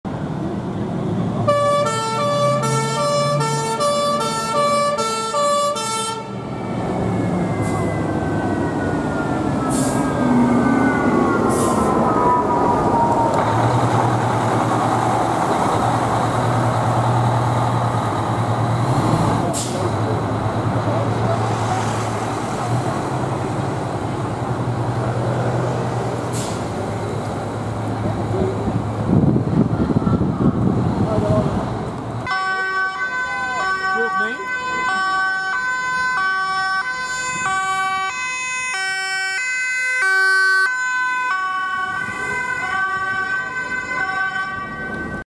Crashtender Brandweer Rotterdam airport en drone-team met 'spoed' vanaf de WereldHavenDagen!
-Dit betreft een SHOWTJE en is geen PRIO 1 rit.-